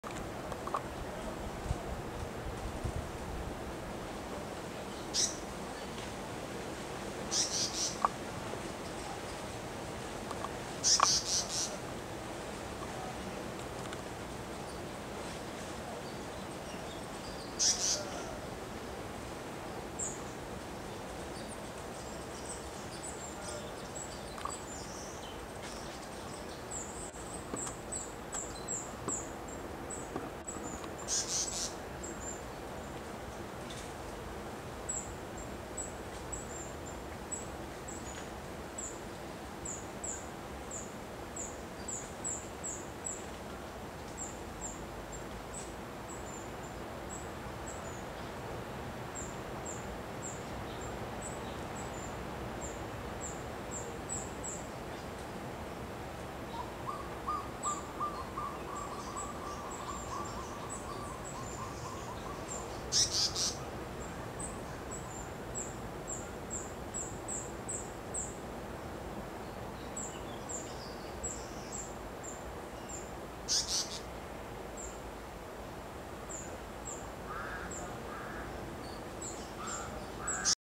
African Paradise Flycatcher (Terpsiphone viridis) Blauwe Paradijsvliegenvanger
Heel beweeglijk tijdens het nemen van een bad in wat water. Volgens mij werd het water nauwelijks aangeraakt maar toch zag ik af en toe wat druppels op het verenkleed. Een hele toer trouwens met zo'n lange staart. Het leek mij het beste om maar op de tak te blijven focussen want de vogel volgen met de telelens wat onmogelijk.